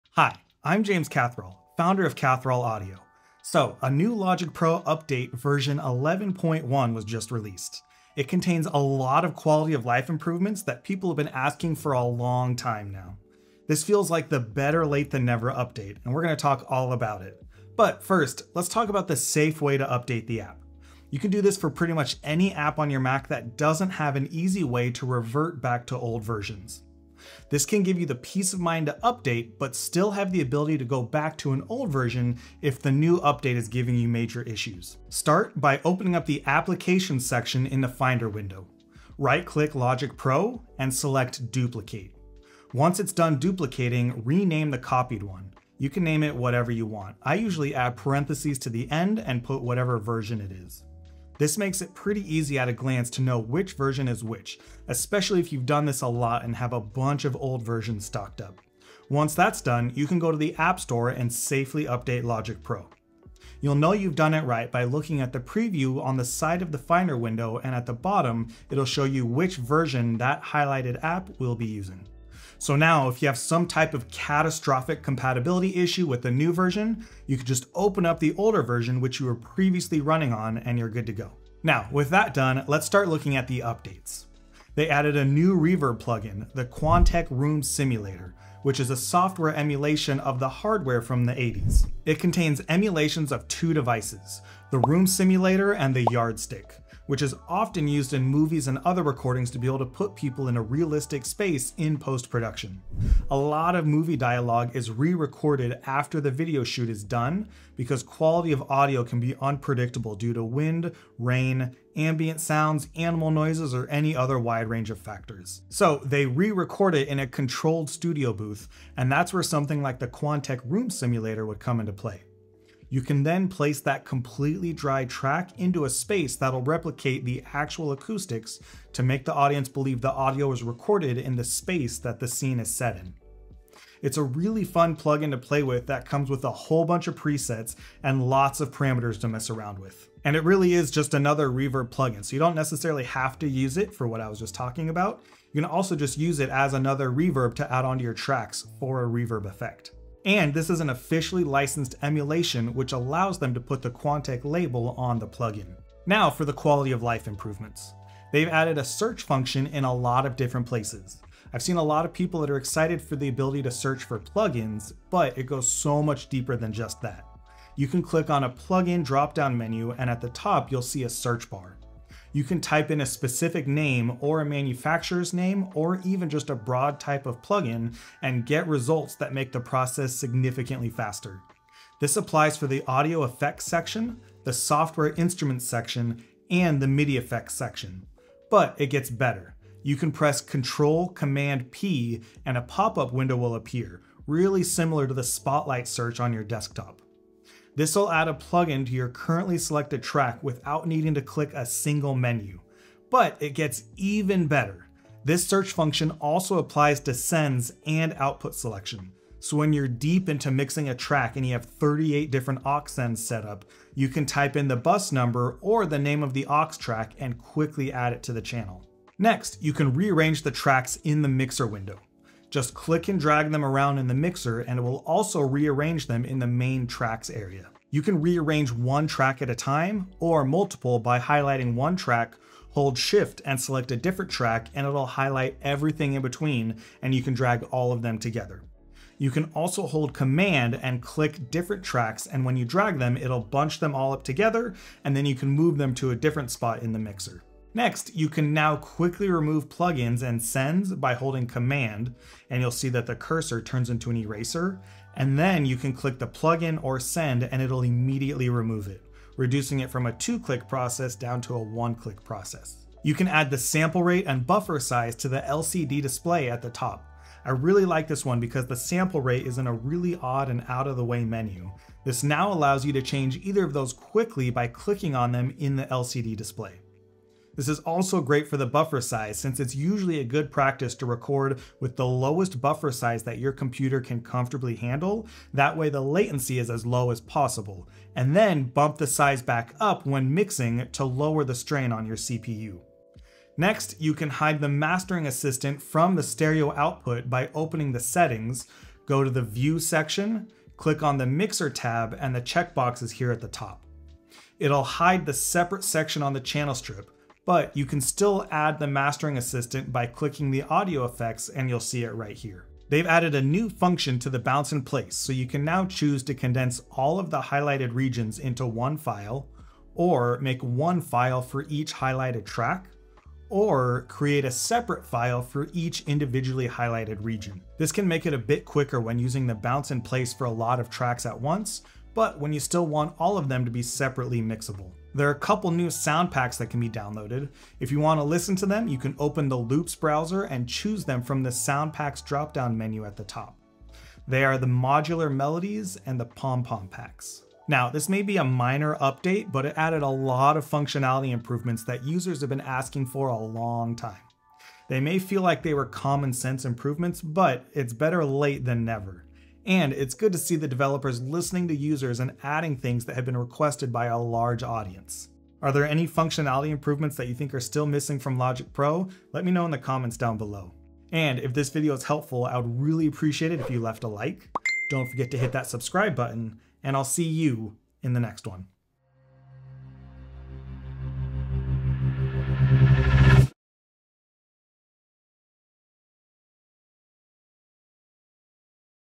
Logic Pro 11.1 Update - Better Late Than Never Improvements!
Logic Pro 11.1 is out! Learn how to update the app safely and find out what's been added and changed!

Chapters
0:00 Intro
0:17 How To Safely Update Logic Pro
1:21 Quantec Reverb Plugin
2:38 Plugin Search
3:23 Sends and Output Routing Search
3:38 Moving Tracks In The Mixer
4:07 Quickly Remove Plugins and Sends
4:22 Add Sample Rate and Buffer Size to LCD Display
4:53 Hide the Mastering Assistant on the Stereo Output
5:13 Bounce In Place Additions
5:34 New Sound Packs
5:47 Final Thoughts